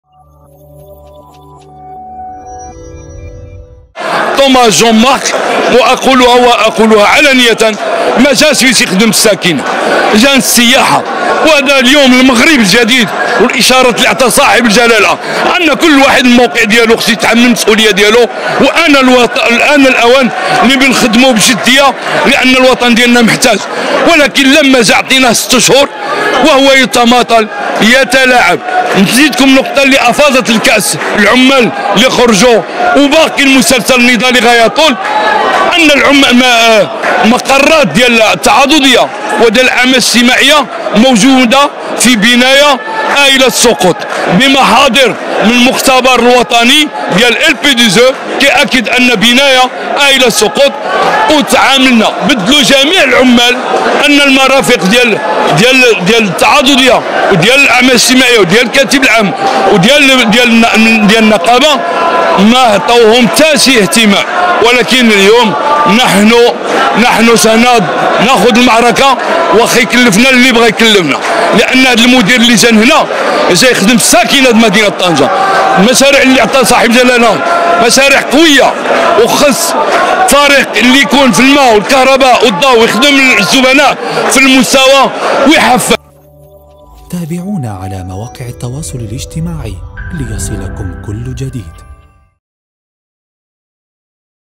فضيحة.. أمانديس تضع مستخدميها وأطرها ببناية ٱيلة للسقوط
طنجاوي- حمزة الرابحي

فجر الكاتب العام لنقابة مستخدمي وأطر "أمانديس" فضيحة مدوية، عندما كشف عن استمرارهم في الاشتغال ببناية آيلة للسقوط بطنجة، رغم المخاطر التي تهدد حياتهم.

وأوضح "رشيد سلمون" الكاتب العام لنقابة مستخدمي أمانديس، في تصريح لموقع "طنجاوي" أثناء الوقفة الاحتجاجية الحاشدة التي نظمها المستخدمون بمرآب السيارات الرئيسي للشركة، أن الإدارة تستمر في الإبقاء على المستخدمين ببناية آيلة للسقوط، والتي خصصتها الشركة للتعاضدية وجمعية الاعمال الاجتماعية، رغم صدور تقرير مختبر وطني يكشف عن المخاطر التي تحدق بالمشتغلين داخل البناية.

وأضاف "سلمون" أن المدير العام للشركة يتحمل مسؤولية كبيرة في ما يعانيه المستخدمون من معاناة قد تهدد حياتهم، بعد استمرار الشركة في تشغيل هذه البناية المهددة بالانهيار في أي لحظة.